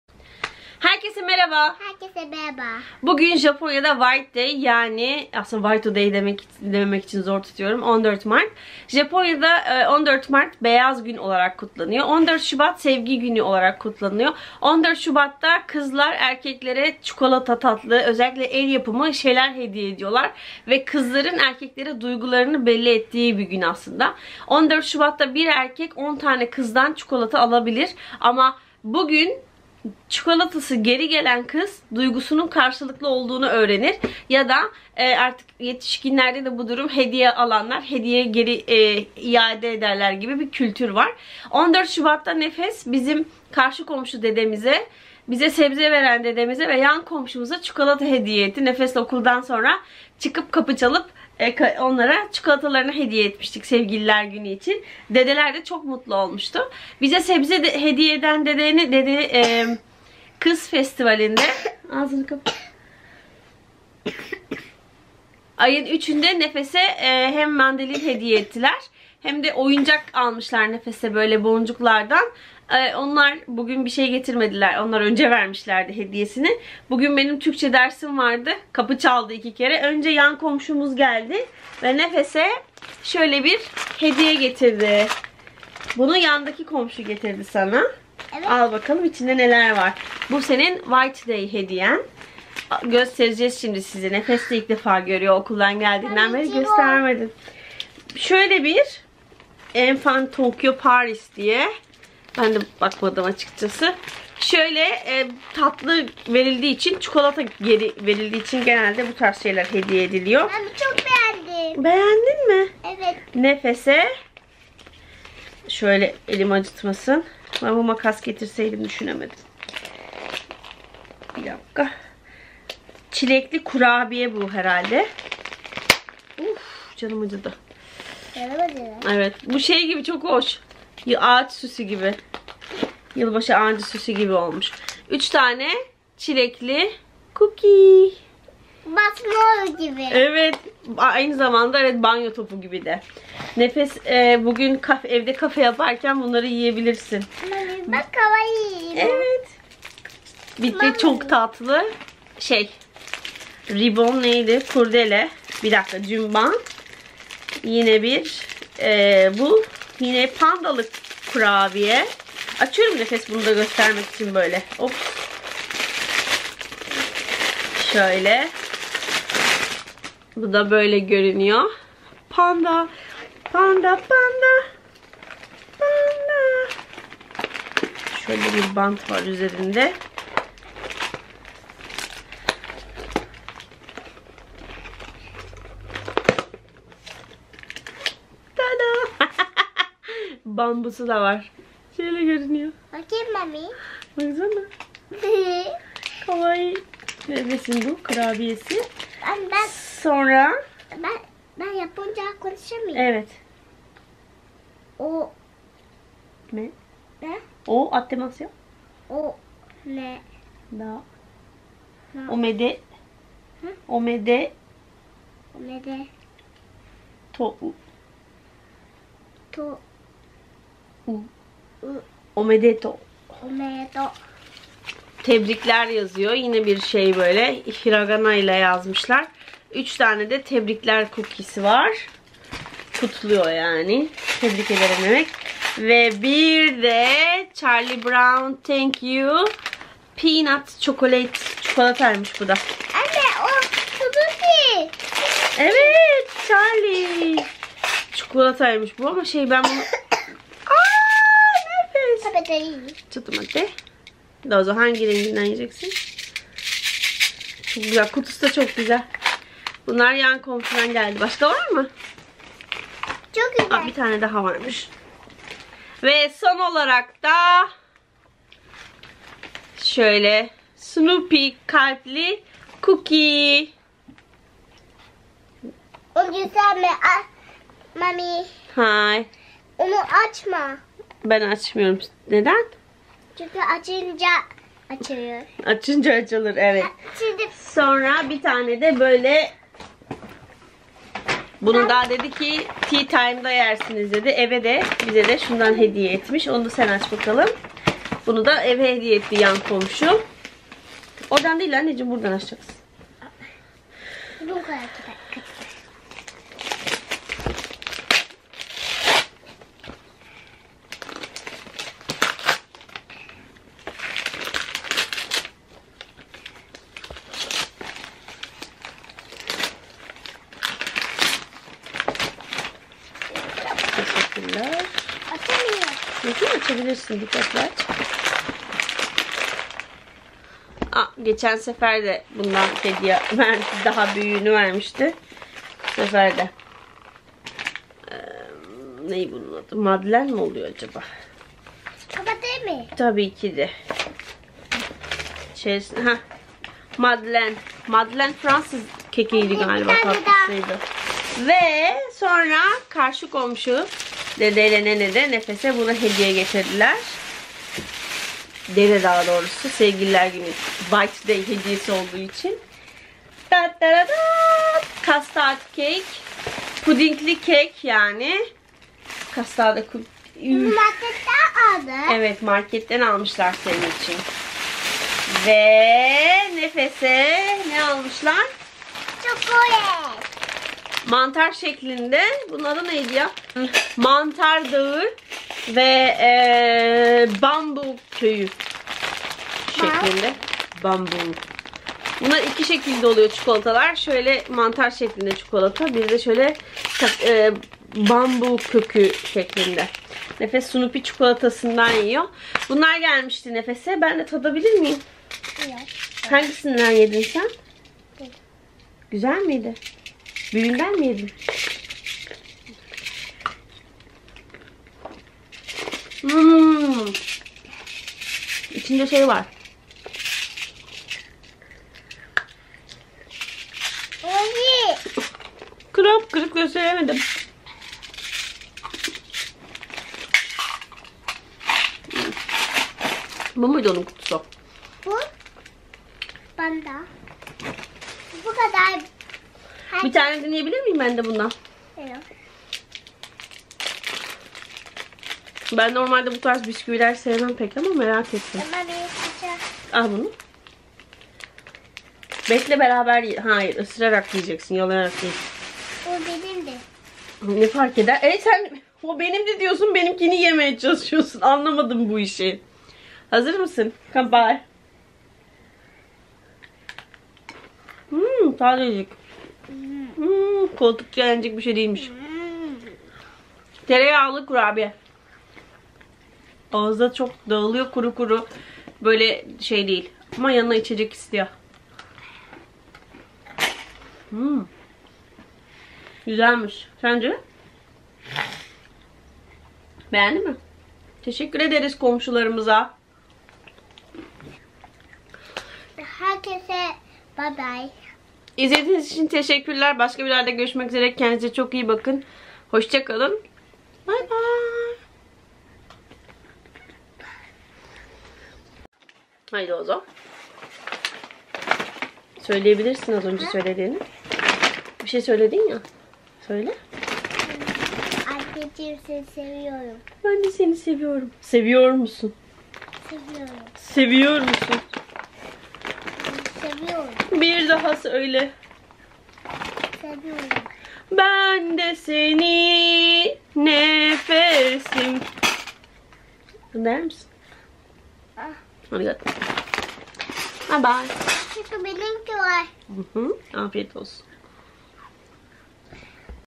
Japon Komşulardan Gelen White Day Hediyeleri - White Day Nedir?
Daha fazla video için kanalıma üye olmayı ve beğen tuşuna basmayı unutmayın :)

Beni bulabileceğiniz diğer sosyal ağlar